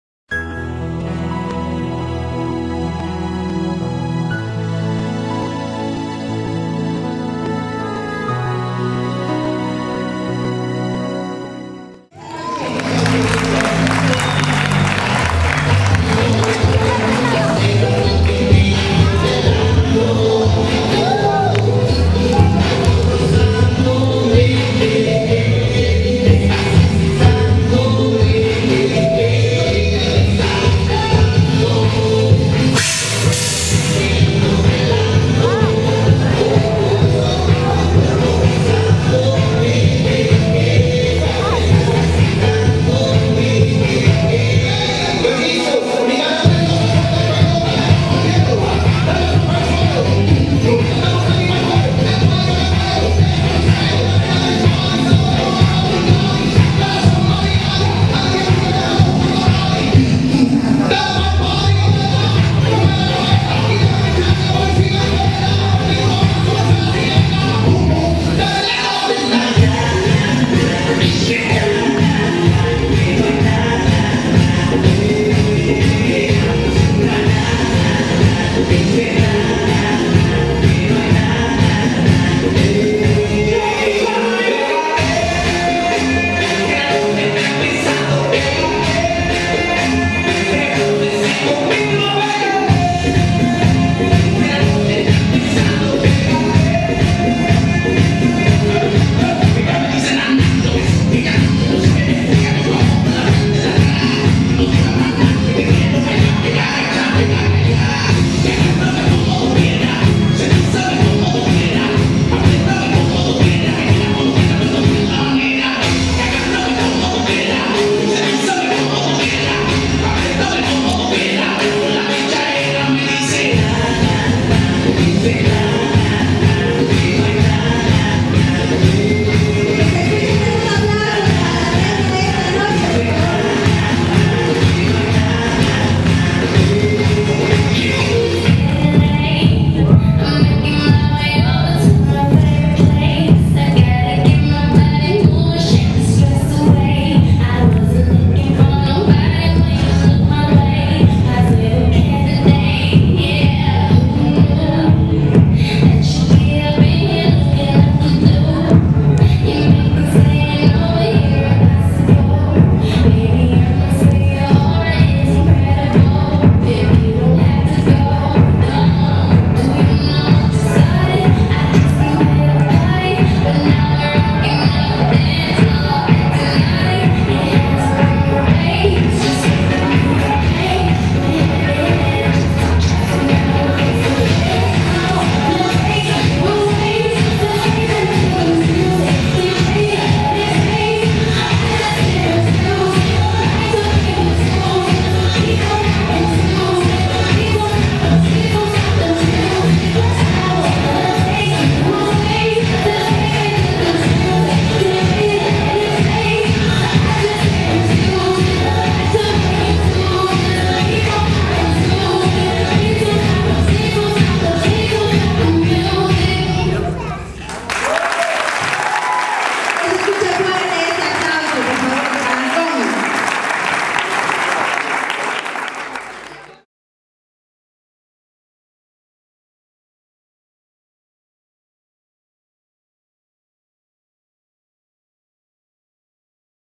Baile Moderno XV Connie
Baile Moderno de 15 años de la Srita Connie Realizado en el Salón Navío en Tepeaca Puebla el 12 de julio de 2009. 55 A Pte. 1303 Prados Agua Azul Puebla Pue. Mexico